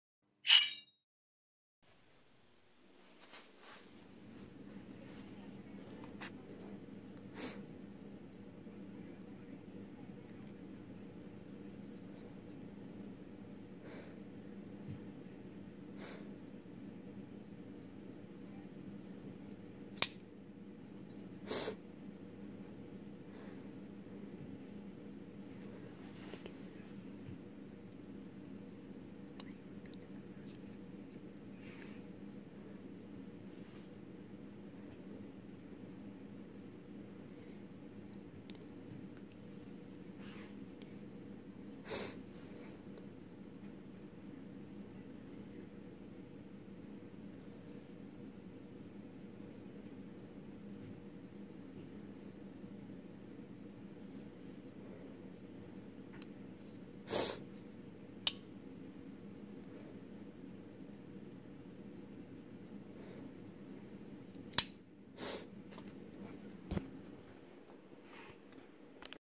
SIG X5 BLACK ALUMINUM EMPEROR BUTTON LOCK FOLDING KNIFE STAINLESS BLADE 36570
4.75" closed. 3.5" dark earth PVD coated CPM-154 stainless spear point blade. Black aluminum handle with black G10 insert. Extended tang. Lanyard hole. Pocket clip. Black nylon zippered storage case. Boxed.